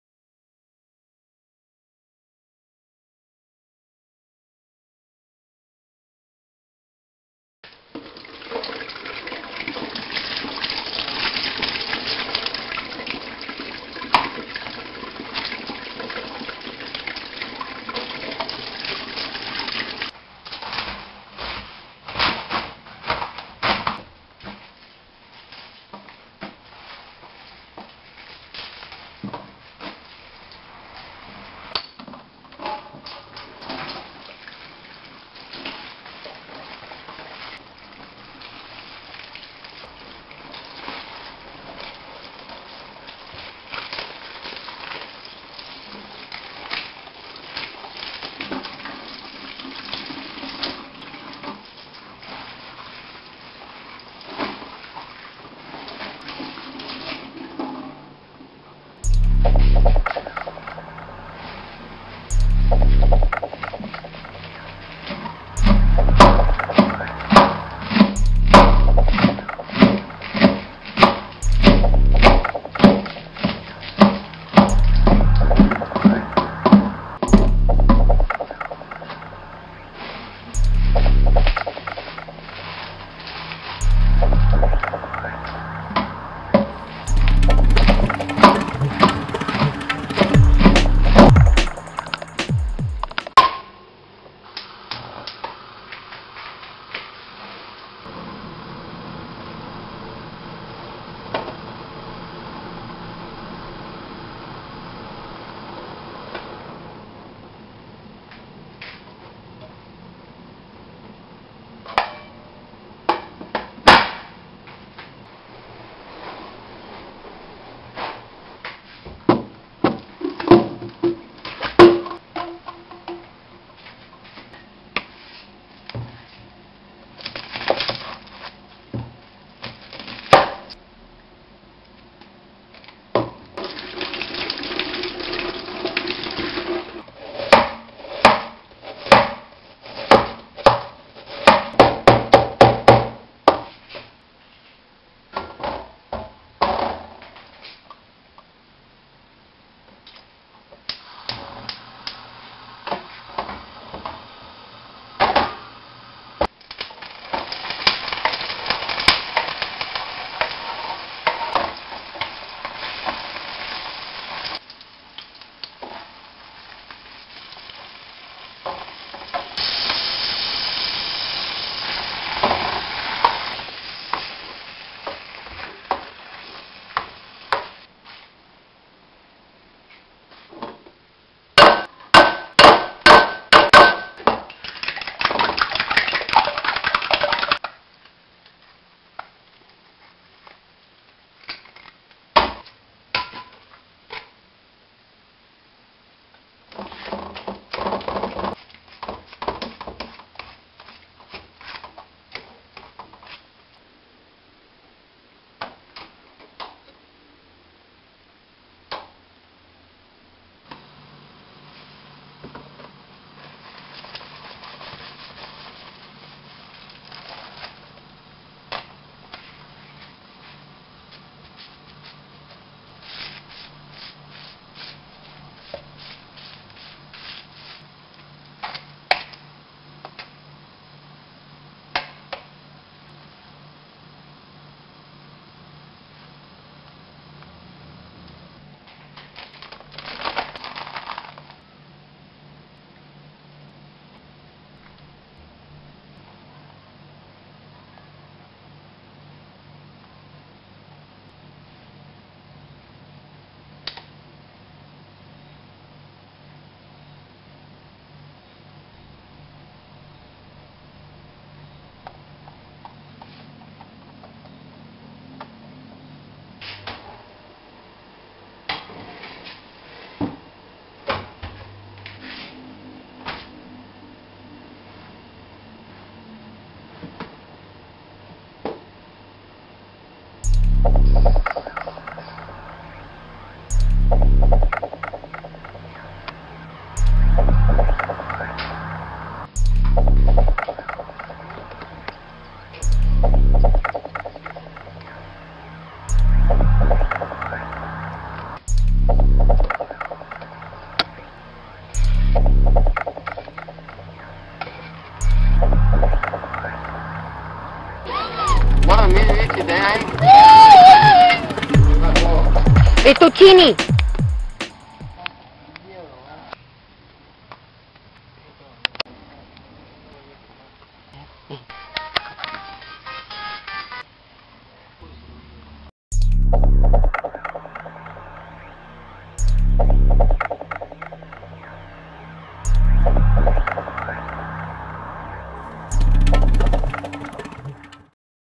Tarta de Acelga (Swiss Chard Tart) o Fettuccine al Ataque! (Fettuccine Attacks!)
A healthy, delicious, easy to make tart. Great for taking on picnics.  Watch me make this tart and the drama that ensues on our family picnic.